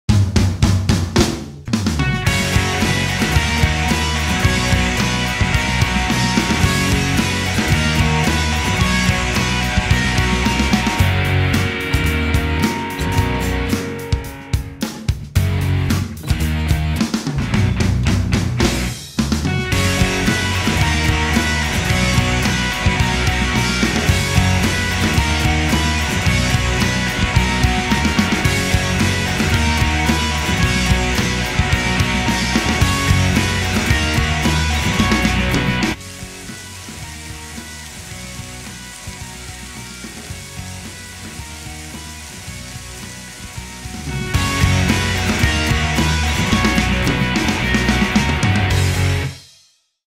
Homemade Corned Beef Hash recipe — Spadea's Fridge
With leftover corned beef from dinner the night before, Bill Spadea made is own homemade corned beef hash inside "Spadea's Fridge".